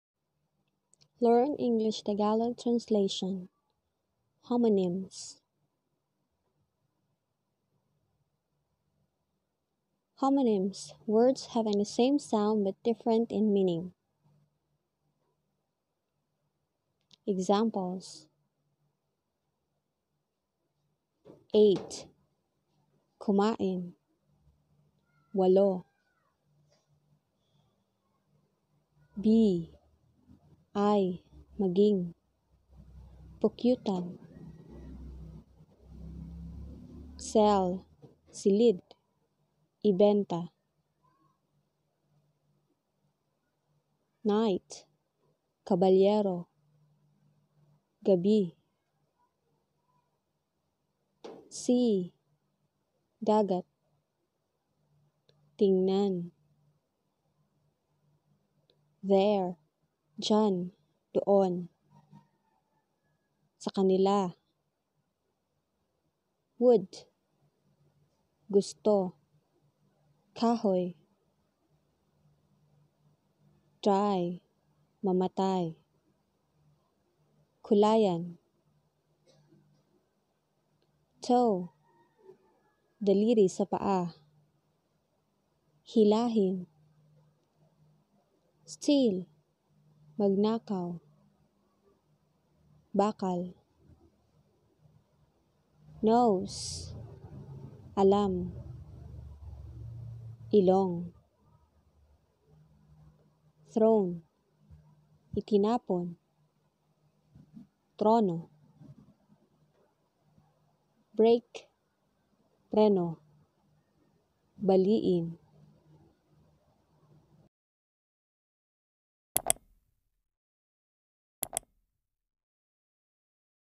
Learn Homonyms (Vocabulary Part 2)
Homonyms-English Tagalog Translation

Homonyms are words having the same sound buy different in meaning.

This video introduces the language English and Tagalog in a way that ang nationality can understand and learn in a basic way.

Learning and embracing new language requires listening skills and mental focus to achieve a good result.

Everyday, this channel uploads new video in which you will learn both English and Tagalog languages highlinghting words, phrases and grammar translations